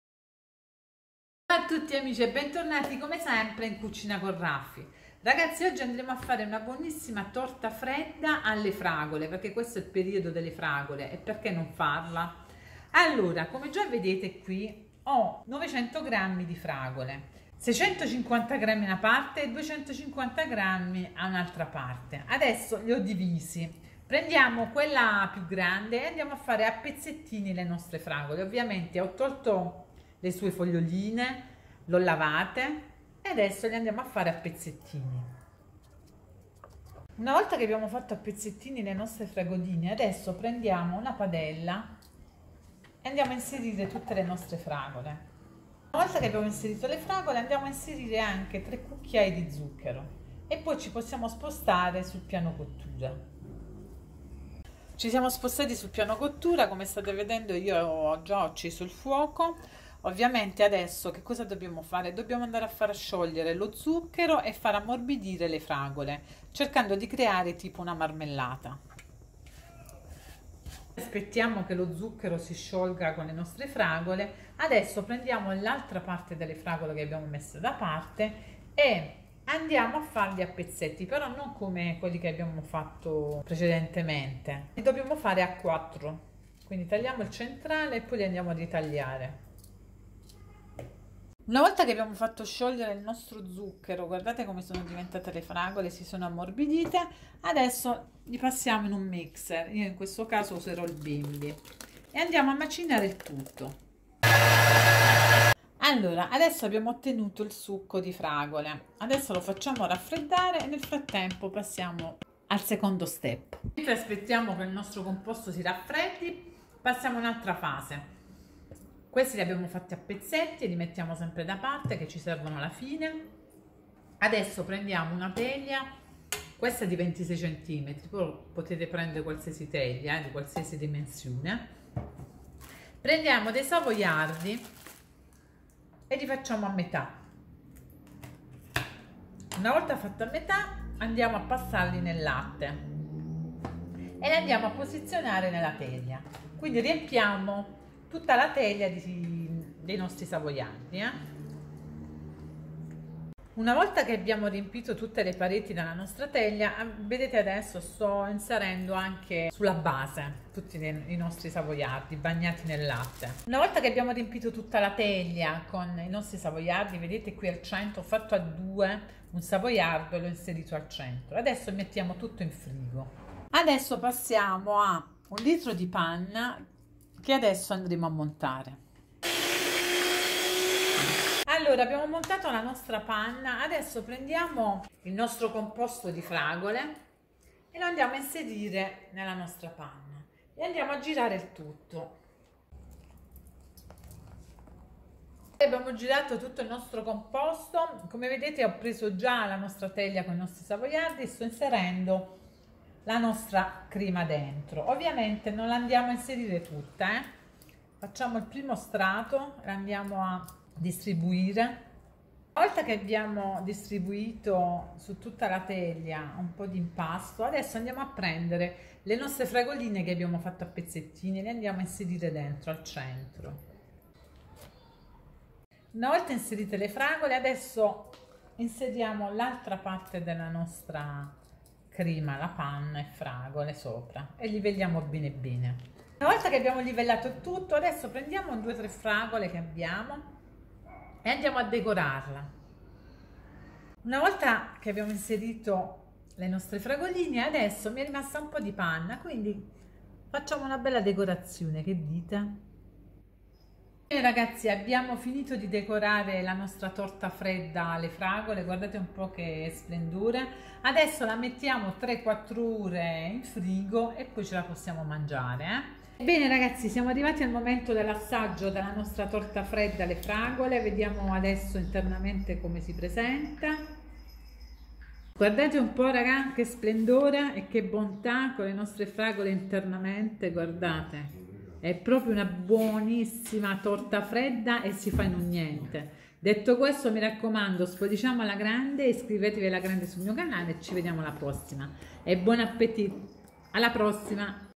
torta fredda alle fragole per la festa della mamma
INGREDIENTI
- 650 g di fragole
- 3 cucchiai di zucchero
- 1 l di panna

- 250 g di fragole

- 1 teglia
- biscotti savoiardi da bagnare nel latte

- fragole per decorare